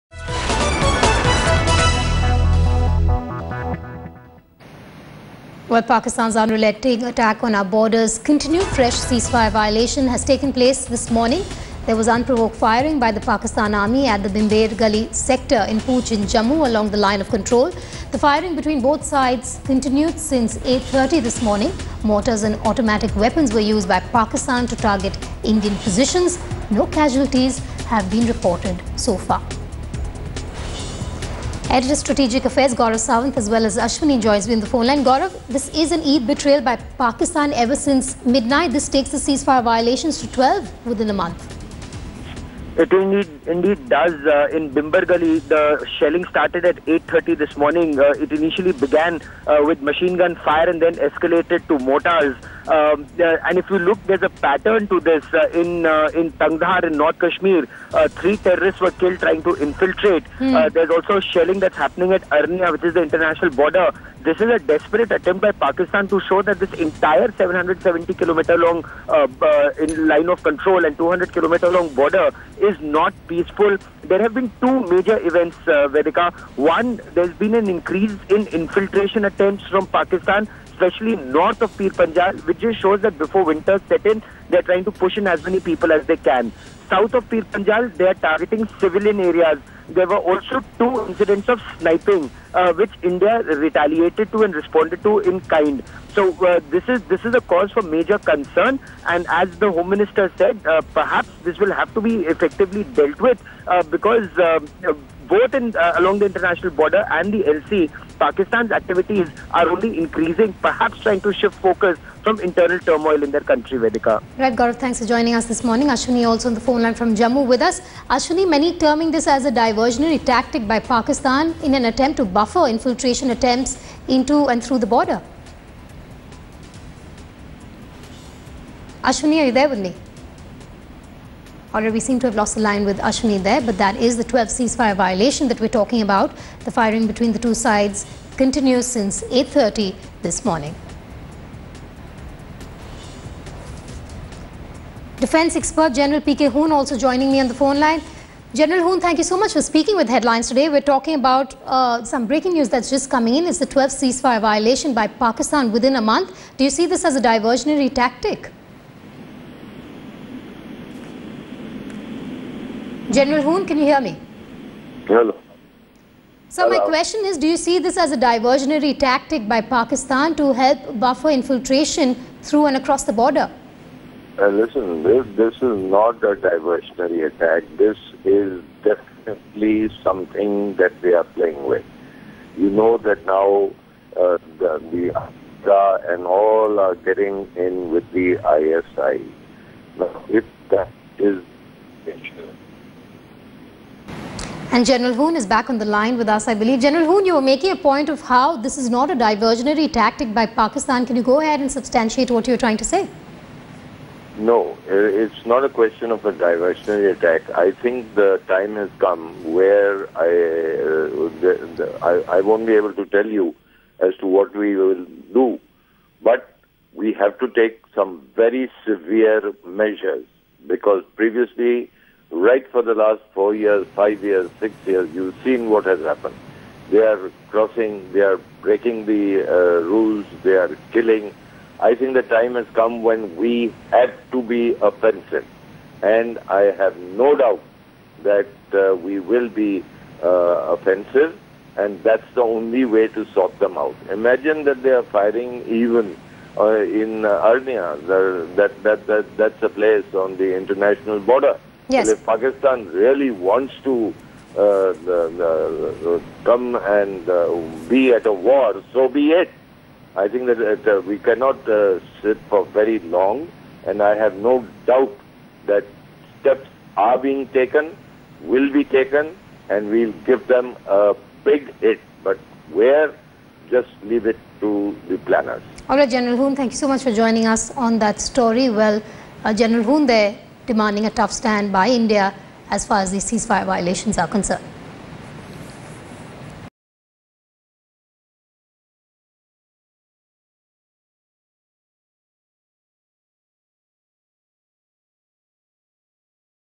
Another ceasefire violation in Bimber Gali, Poonch, by Pakistan
Pakistan aggresses and we react. This seems to be a pattern that neither side seems to be able to break nor want to change. This morning saw another ceasefire violation from the Pak side of the LoC and the Indian side has been returning fire. Check out this clip to find out more.